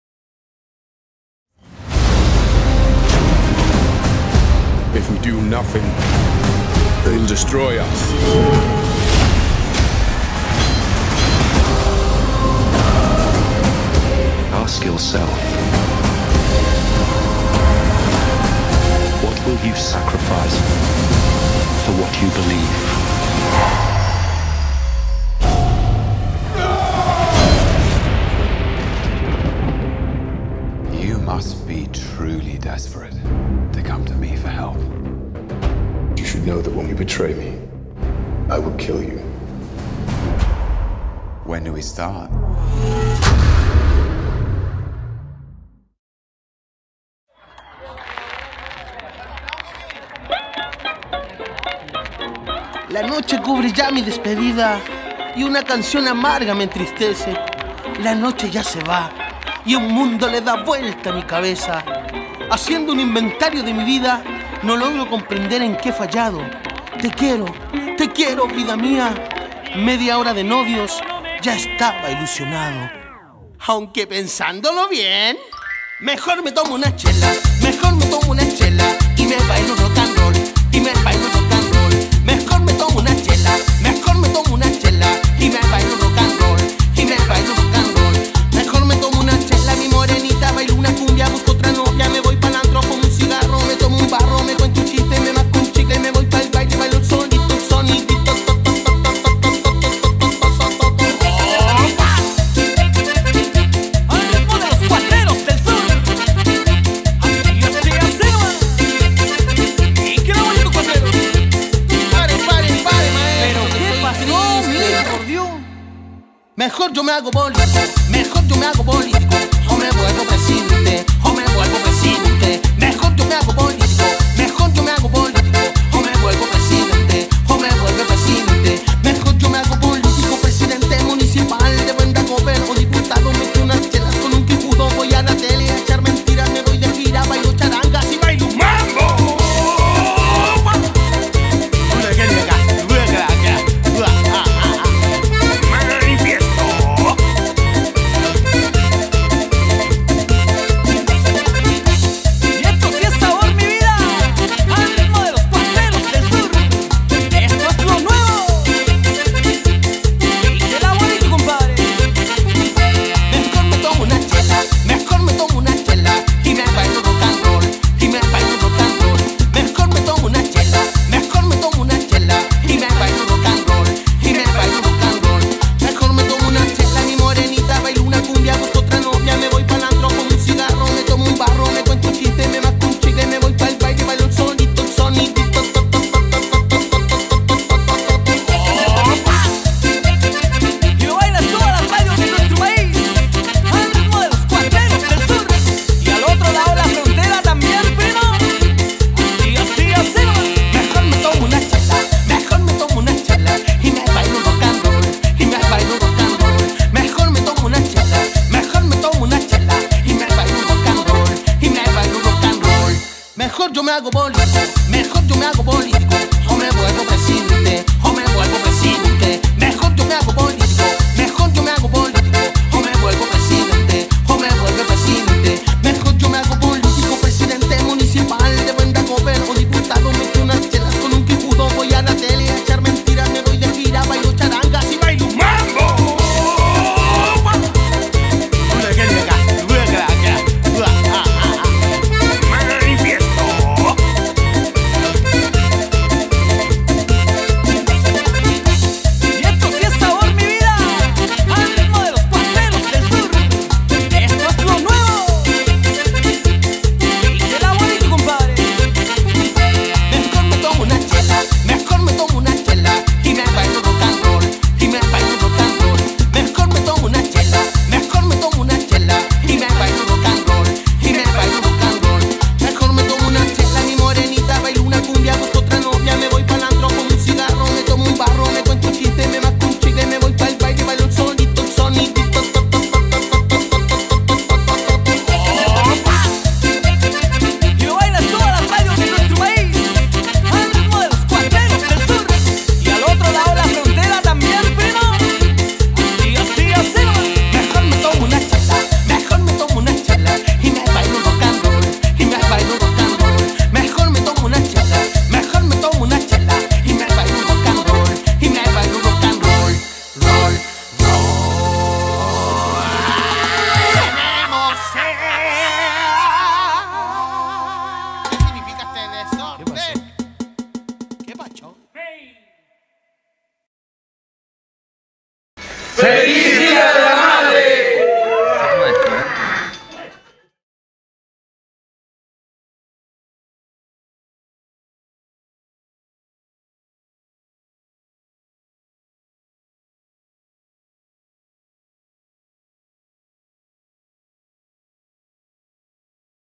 Video Dia de la Madre PEUMO 2013, LOS WATERZ - Tema de Los Cuatreros del Sur
Video grabado y dedicado para todas las madres, en especial para aquellas madres de esta hermosa tierra llamada Peumo.
FELIZ DÍA para todas ellas, esperamos que lo disfruten y podamos a través de este medio, llevarles un momento de alegría...